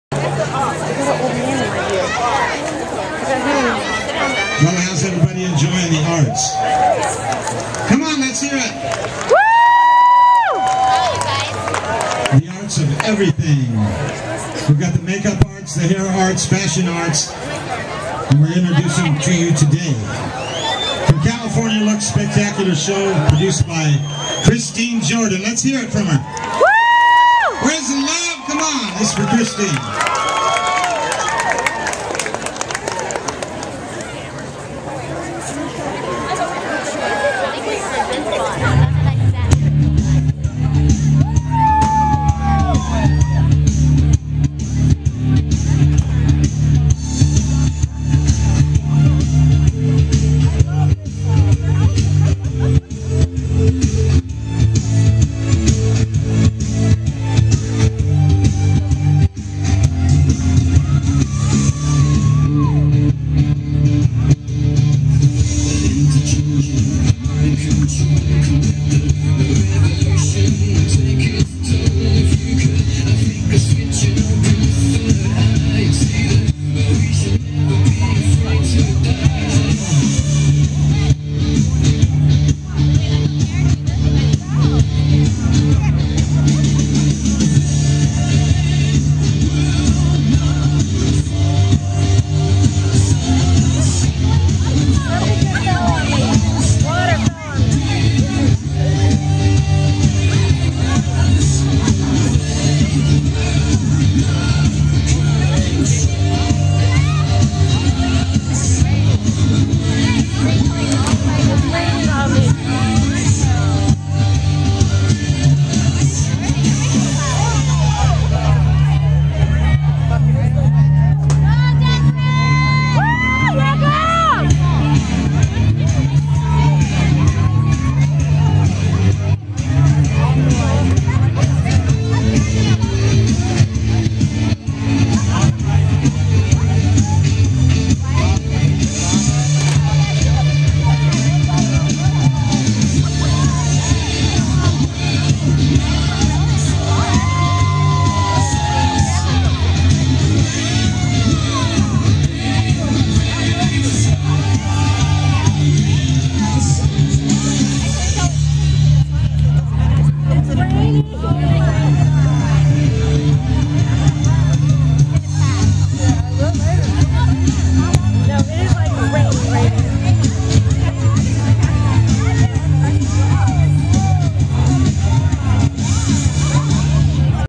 GW dancers at Arts Exposed Fashion Show
Kelsie and some of the Golden West dancers performing at the Arts Exposed Fashion Show on October 16th, 2010